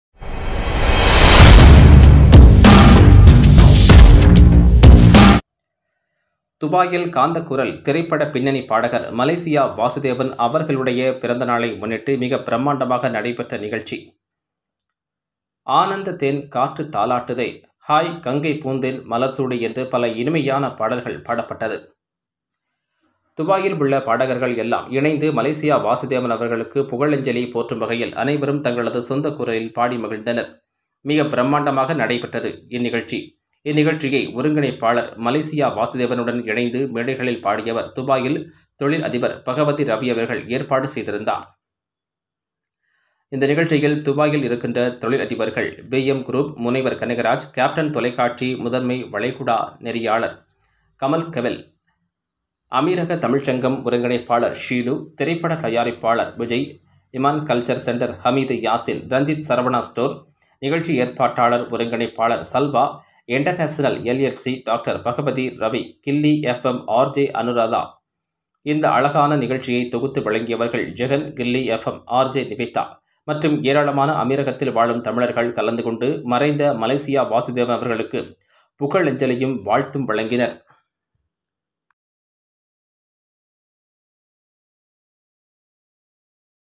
காந்தக்குரல் பாடகர் மலேசியா வாசுதேவன்பிறந்தநாளைமுன்னிட்டுஅவர்பாடலைப்பாடிஅசத்திய ஆர்கெஸ்ட்ரா துபாயில்
Dubai gulf country Malaysia Vasudevan birthday celebrate orchestra#